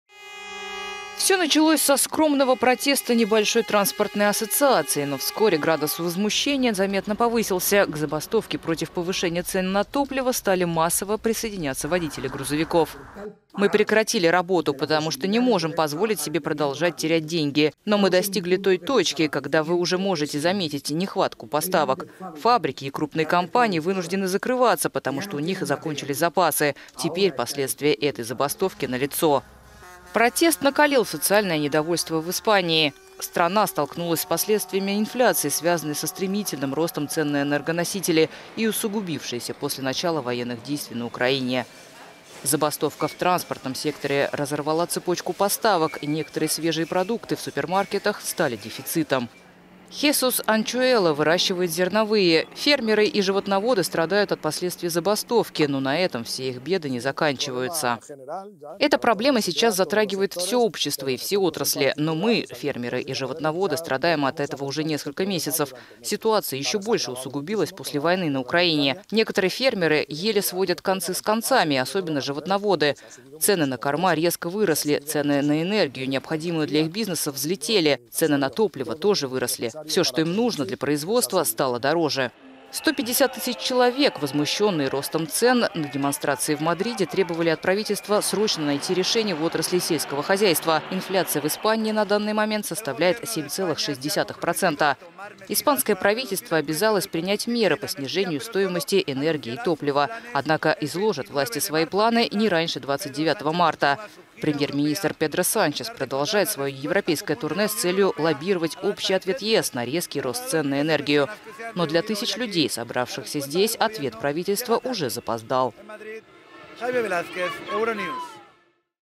В Мадриде прошла многотысячная акция протеста против роста цен на топливо
Масштаб протеста в Испании резко увеличился после присоединения к нему дальнобойщиков. В воскресенье в Мадриде прошла многотысячная демонстрация во главе с фермерами